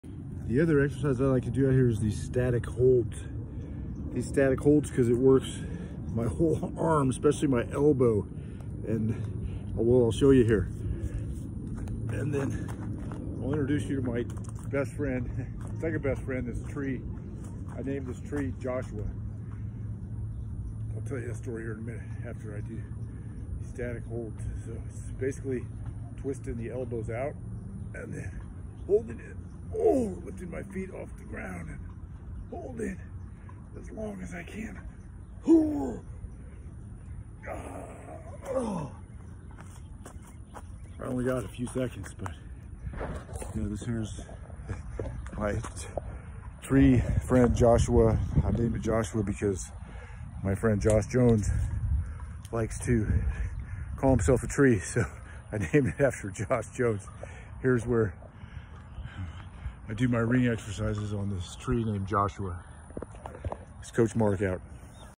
Static Holds with Gymnastics Rings for Straight Arm Strength!
I can feel this throughout my whole body, but especially good for shoulder, arm, elbow, forearm, and wrist strength!